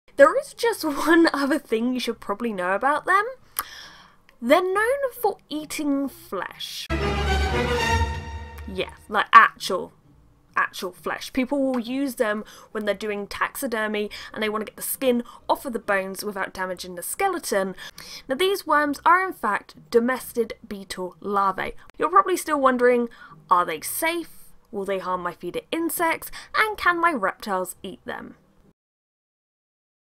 Meet My Flesh-Eating Pet Worms
Yes, you read that right, these little fuzzy worms come in handy!